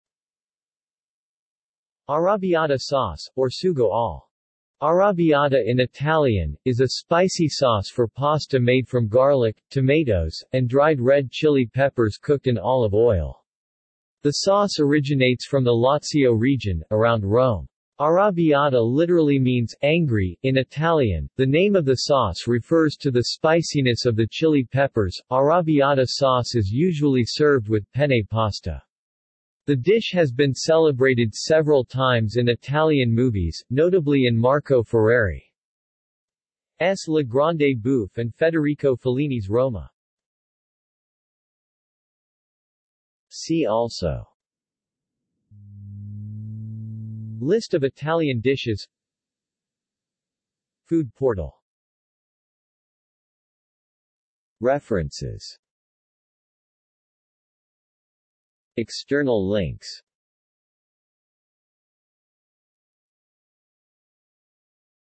Arrabbiata sauce | Wikipedia audio article
This is an audio version of the Wikipedia Article:
Arrabbiata sauce

SUMMARY
Arrabbiata sauce, or sugo all'arrabbiata in Italian, is a spicy sauce for pasta made from garlic, tomatoes, and dried red chili peppers cooked in olive oil. The sauce originates from the Lazio region, around Rome. Arrabbiata literally means "angry" in Italian; the name of the sauce refers to the spiciness of the chili peppers.Arrabbiata sauce is usually served with penne pasta. The dish has been celebrated several times in Italian movies, notably in Marco Ferreri's La Grande Bouffe and Federico Fellini's Roma.